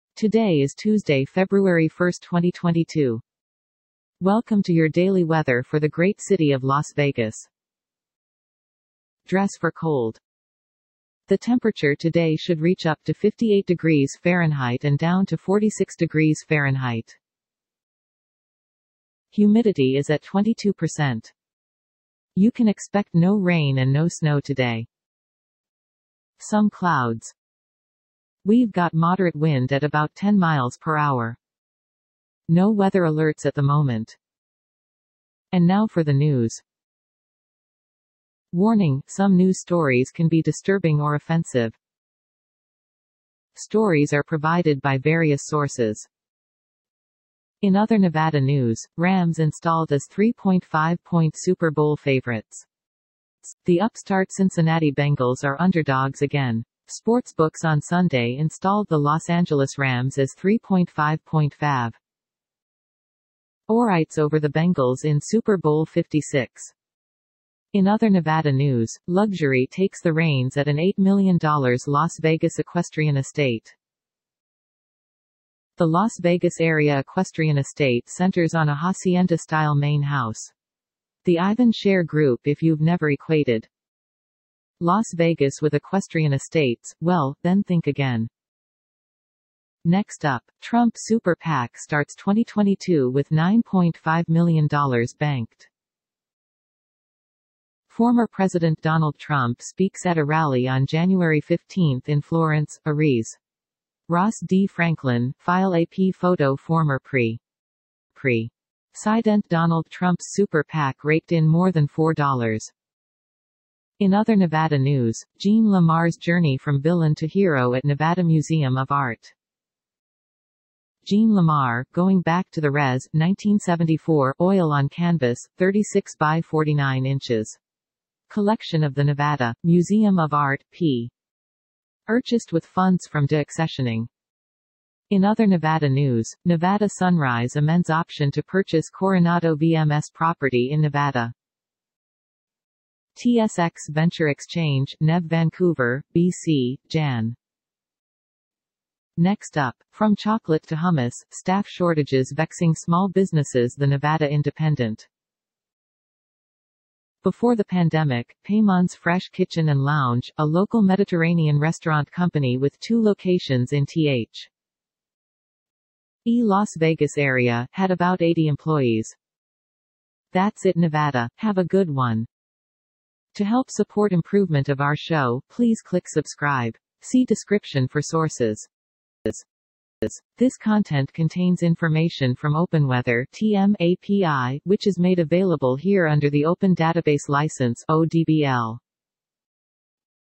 News and Weather Forecast for Tuesday February 01, 2022 in Las Vegas, NV
The daily news+weather for Nevada.Weather Forecast for Las Vegas on Monday January 2022. This is an autogenerated weather and news report.

Story #1 titled: Rams installed as 3.5-point Super Bowl favorites
From David Purdum
At ESPN

Story #2 titled: Luxury Takes The Reins At An $8-Million Las Vegas Equestrian Estate
From Lauren Beale, Contributor,
 Lauren Beale, Contributor

Story #3 titled: Trump super PAC starts 2022 with $9.5M banked
From Brittany Gibson
At Politico
Image: None

Story #4 titled: Jean LaMarr’s Journey From Villain To Hero At Nevada Museum Of Art
From Chadd Scott, Contributor,
 Chadd Scott, Contributor

Story #5 titled: Nevada Sunrise Amends Option to Purchase Coronado VMS Property in Nevada
From CNW Group
At Yahoo Entertainment

Story #6 titled: From chocolate to hummus, staff shortages vexing small businesses - The Nevada Independent
From Jannelle Calderon
At The Nevada Independent